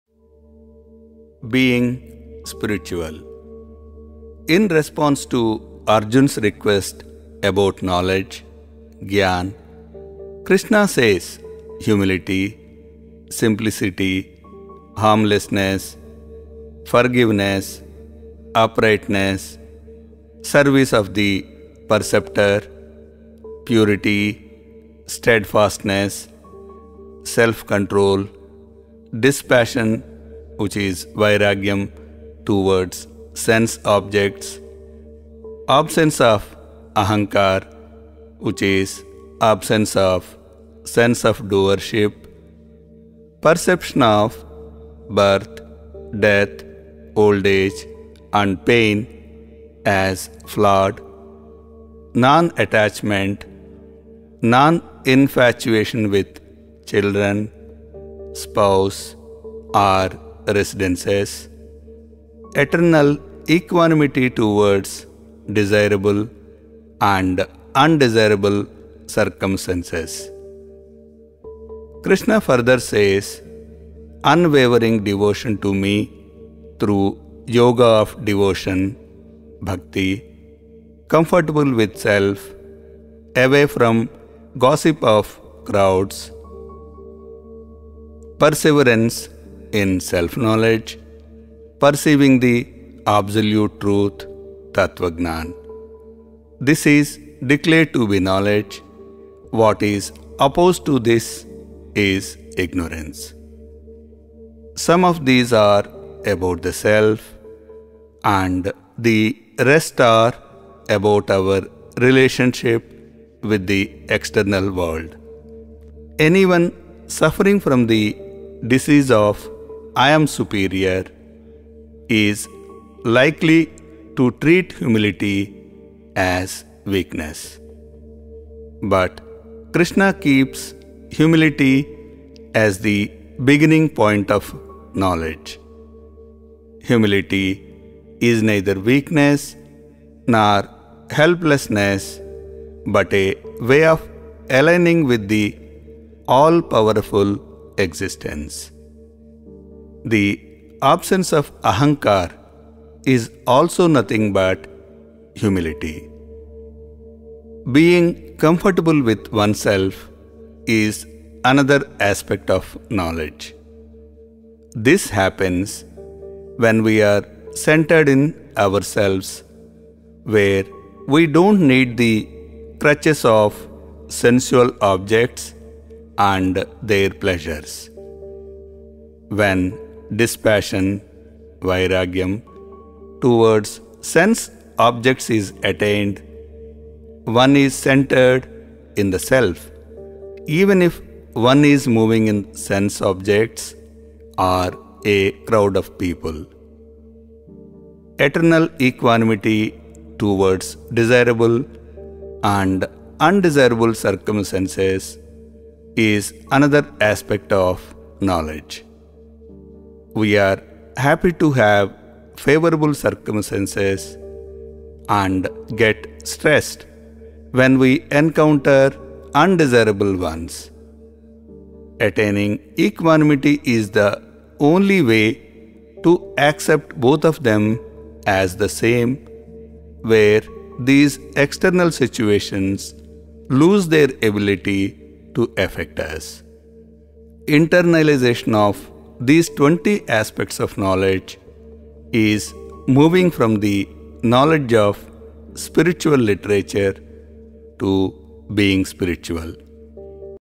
𝐁𝐞𝐢𝐧𝐠 𝐒𝐩𝐢𝐫𝐢𝐭𝐮𝐚𝐥 | 𝐄𝐩𝐢𝐬𝐨𝐝𝐞 - 𝟏𝟗𝟏 | 𝐆𝐢𝐭𝐚 𝐀𝐜𝐡𝐚𝐫𝐚𝐧
In response to Arjun's request about knowledge (𝙜𝙮𝙖𝙣) , Krishna says, "Humility, simplicity, harmlessness, forgiveness, uprightness, service of the preceptor, purity, steadfastness, self-control (13.8). Dispassion (𝙫𝙖𝙞𝙧𝙖𝙜𝙮𝙖𝙢) towards sense objects, absence of 𝙖𝙝𝙖𝙣𝙠𝙖𝙖𝙧 (I am doer), perception of birth, death, old age and pain as flawed (13.9). Non-attachment, non-infatuation with children, spouse or residences, eternal equanimity towards desirable and undesirable circumstances" (13.10).

Krishna further says, "Unwavering devotion to Me through yoga of devotion (𝙗𝙝𝙖𝙠𝙩𝙞) , comfortable with self, away from gossip of crowds (13.11). Perseverance in self-knowledge, perceiving the absolute truth ( tatva gyan) . This is declared to be knowledge, what is opposed to this is ignorance" (13.12). Some of these are about the self and the rest are about our relationship with the external world.

Anyone suffering from the disease of 'I am superior', is likely to treat humility as weakness. But Krishna keeps humility as the beginning point of knowledge. Humility is neither weakness nor helplessness, but a way of aligning with the all powerful existence. The absence of 𝙖𝙝𝙖𝙣𝙠𝙖𝙖𝙧 is also nothing but humility.

Being comfortable with oneself is another aspect of knowledge. This happens when we are centered in ourselves where we don't need the crutches of sensual objects and their pleasures. When dispassion (𝙫𝙖𝙞𝙧𝙖𝙜𝙮𝙖𝙢) towards sense objects is attained, one is centred in the self even if one is moving in sense objects or a crowd of people.

Eternal equanimity towards desirable and undesirable circumstances is another aspect of knowledge. We are happy to have favourable circumstances and get stressed when we encounter undesirable ones. Attaining equanimity is the only way to accept both of them as the same where these external situations lose their ability to affect us. Internalisation of these twenty aspects of knowledge is moving from the knowledge of spiritual literature to being spiritual.